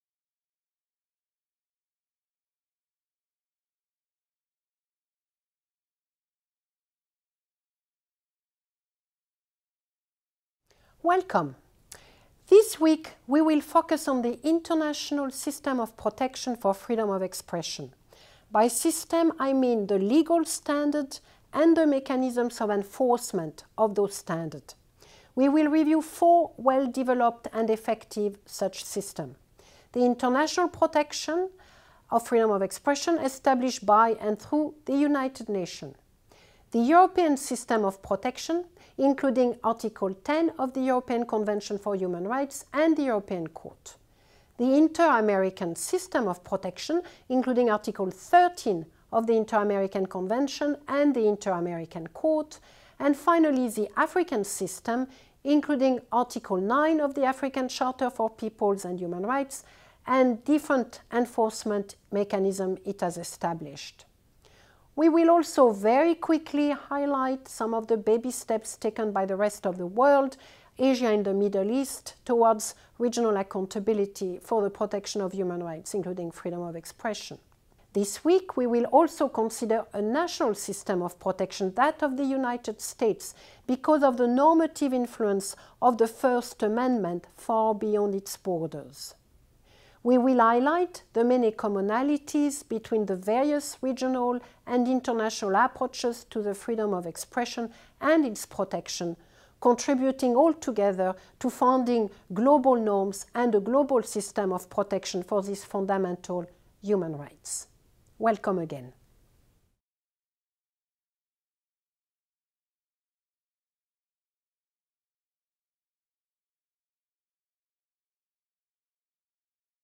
MOOC FOE1x | 2.1.1 Introduction and Overview | International Norms on Freedom of Expression
2.1.1 Introduction and Overview | International Norms on Freedom of Expression
MOOC FOE1x | Freedom of Expression in the Age of Globalization
ColumbiaX | Agnès Callamard

Are you interested in understanding freedom of expression and information? Do you want to learn how it is defined under international law, who abuses it and how it is protected on or off-line?

Adopting a global standpoint, this course will present the international human rights norms and laws that define free speech and the global jurisprudence that has developed to safeguard it. It will explore political expression, press freedom and freedom of expression on the Internet. It will outline the legitimate restrictions to freedom of expression, and discuss its most pressing challenges and violations globally.

Taught with the support of journalists, human rights defenders and press freedom activists from around the world, this course is a formidable introduction to a complex fundamental right, at the heart of today’s many global challenges, from the Internet to national security, freedom of religion and privacy.